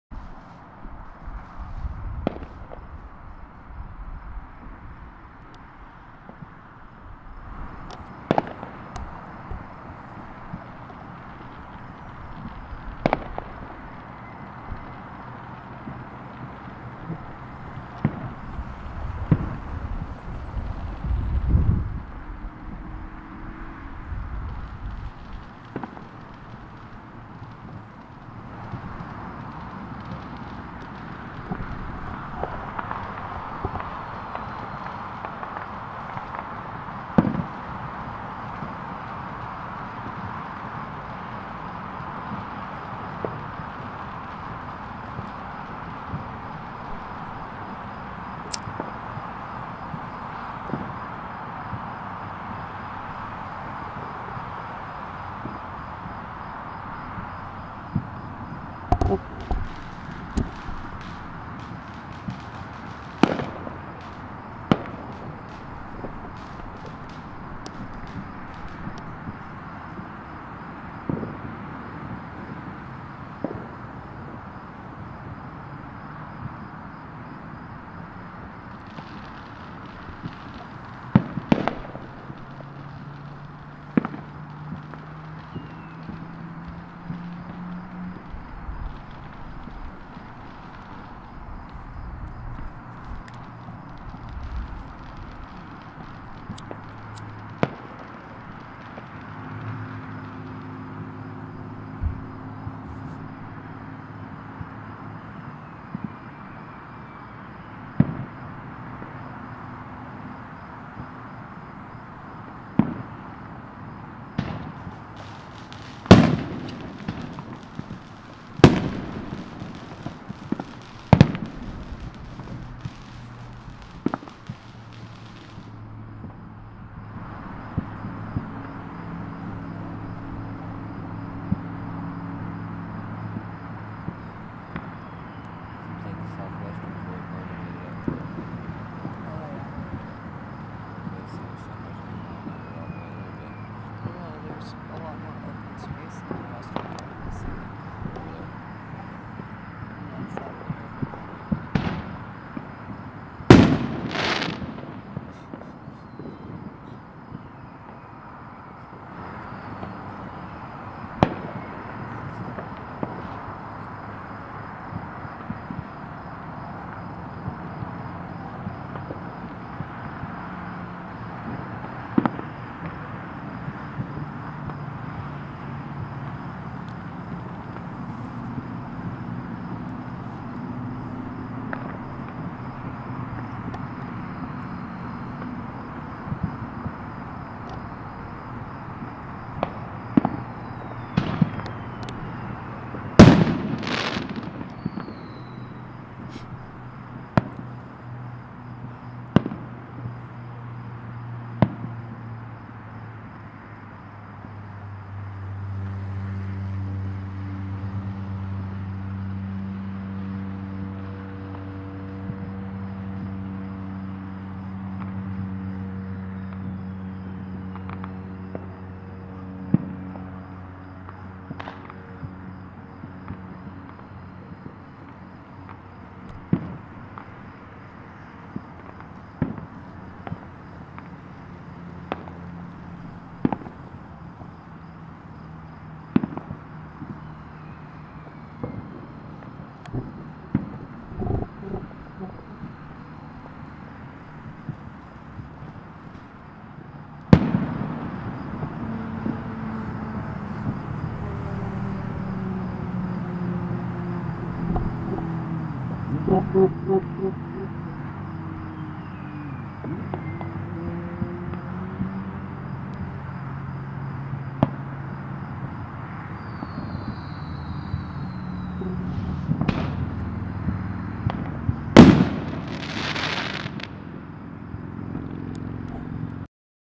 4 Minutes of Spectacular Fireworks 4th of July, 2016 Albuquerque, New Mexico, USA Part 1
Fireworks over the Rio Grande River and Downtown Albuquerque, NM, USA looking West from I-40, July 4th, 2016.
Western Esoteric meets Eastern Shamanism
New Age New Tech from Ancient Sages